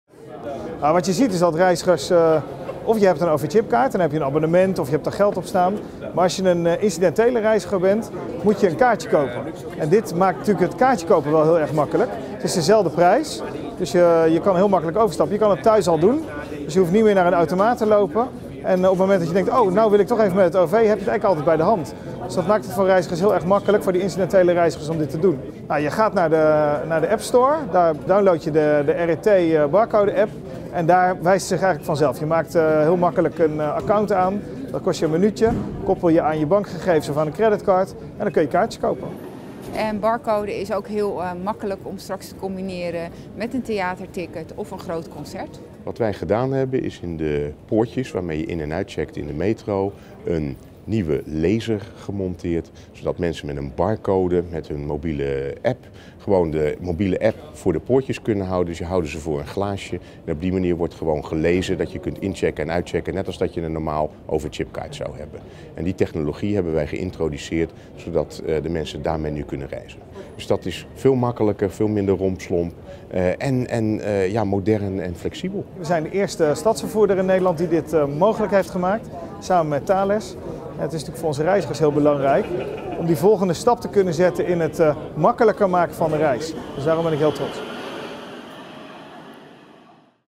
RET Barcode app
De RET introduceert als eerste de Barcode app waarmee reizigers met hun mobiel in en uit kunnen checken bij de metro. Een eerste stap op weg naar meer gebruiksgemak voor reizigers. Eerder konden reizigers met de app al op zicht instappen bij de tram en de bus, nu is de techniek zover dat de app gescand kan worden door de metropoortjes.